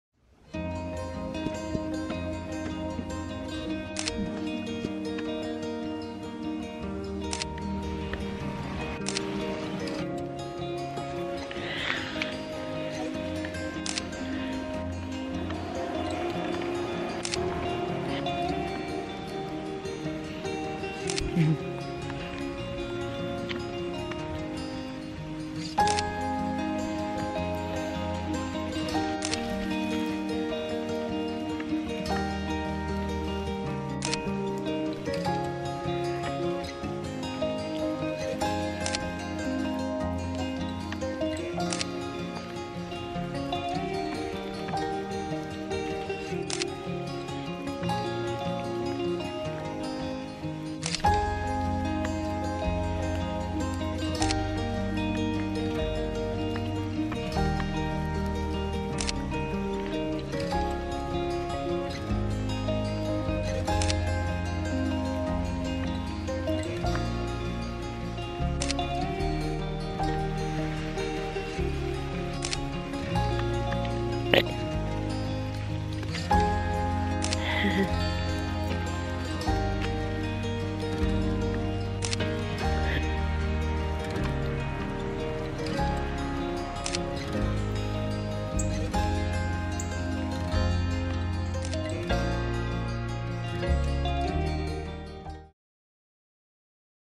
Selfie's In The Rain!
I decided to take some selfie's while it was lightly raining.